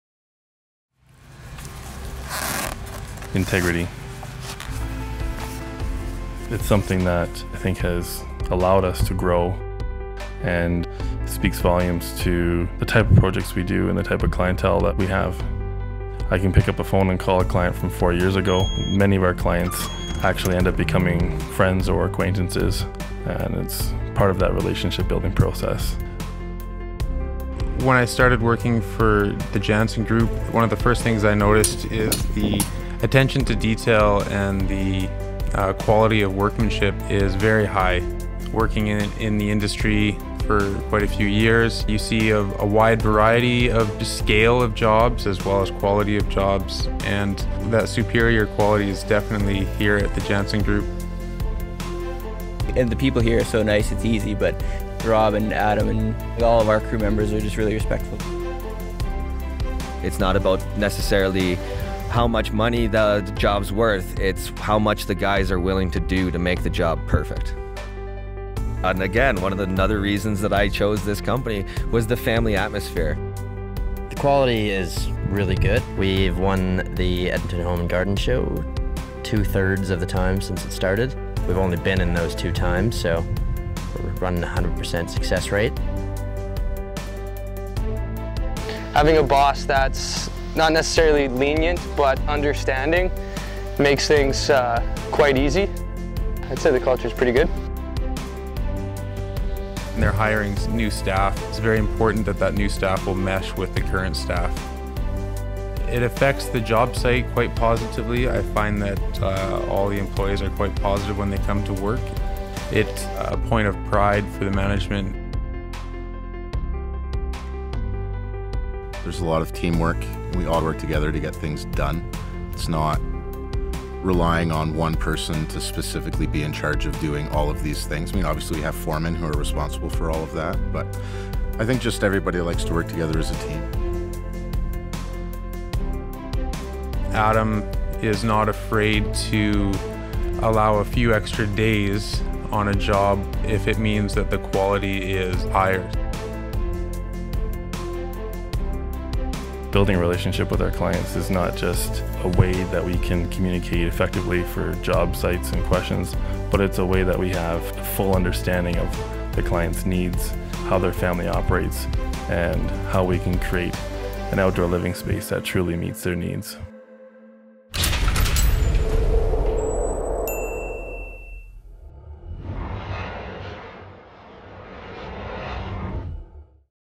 The Jansen Group - Our Staff
At The Jansen Group we strive to recruit only the very best staff. Our team is made up of a diversified group of men and women who love what they do. This short video provides some insight into what it's like to work at The Jansen Group.